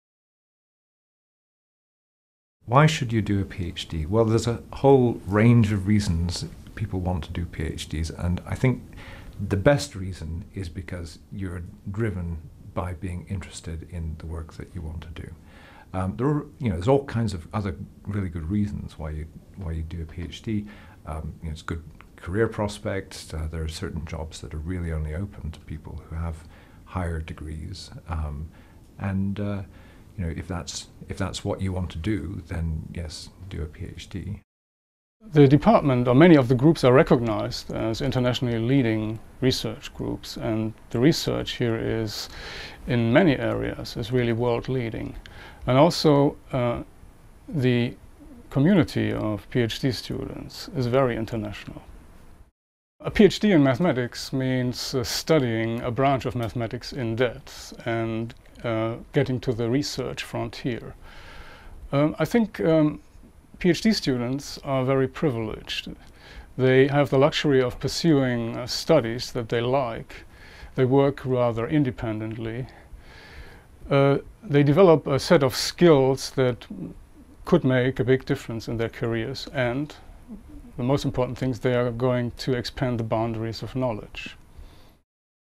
Why do a PhD - what is it and what does it entail
If you are interested in learning more about why you should do a PhD, what a PhD is and what it entails then watch this video.